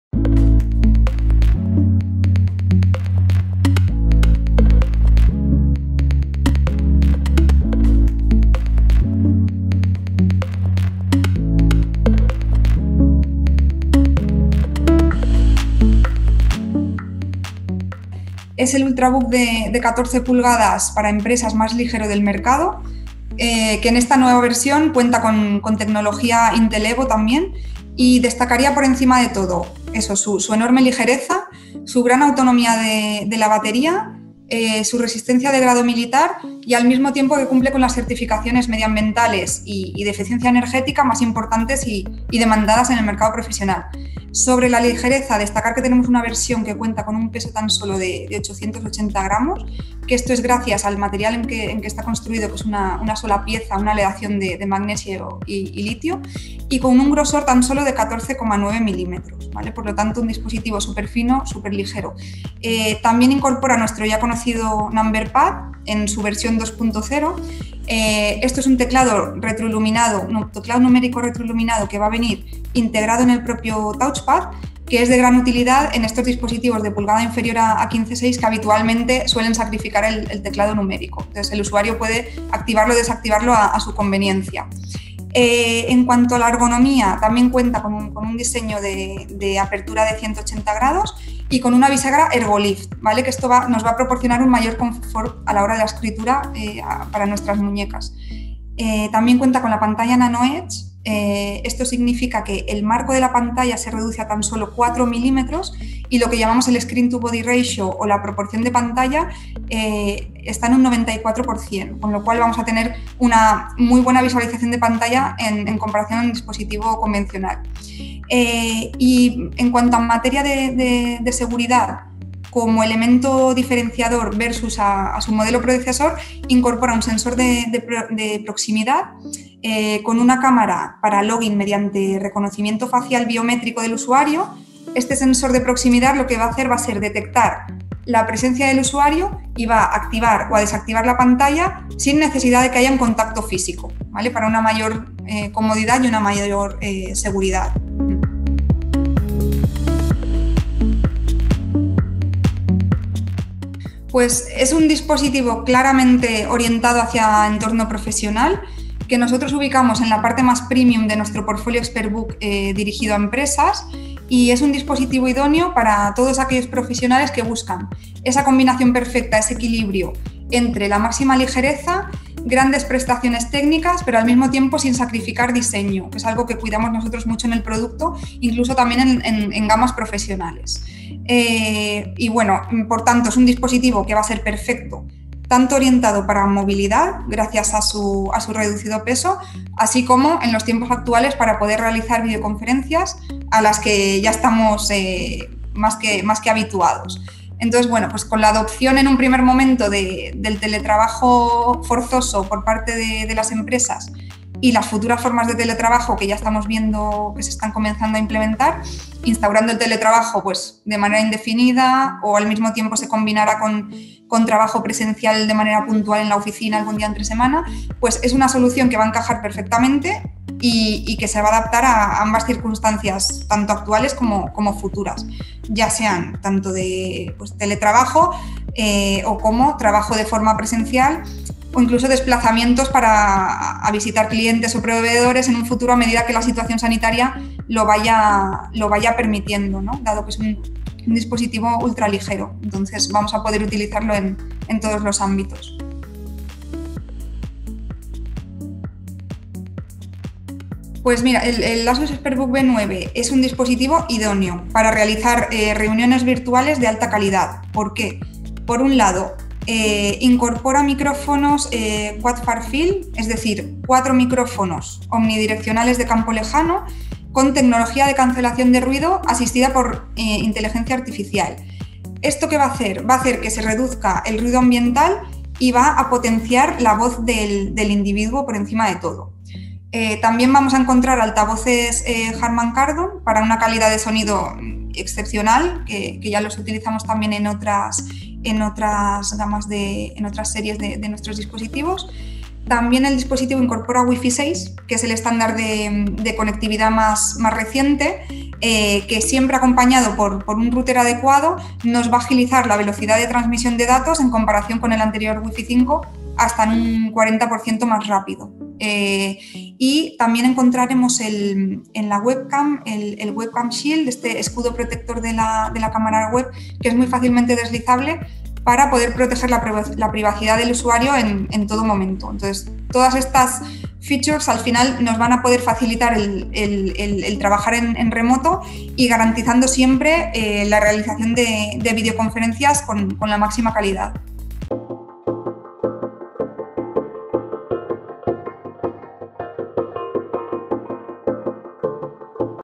ASUS EXPERTBOOK B9 el portátil de 14 para empresa más ligero del mercado
Sara García, product manager en Asus, habla de los factores diferenciales del ExpertBook B9, un equipo de menos de un kilo, pero muy resistente y robusto al mismo tiempo, y que está dirigido a profesionales de alto rango y directivos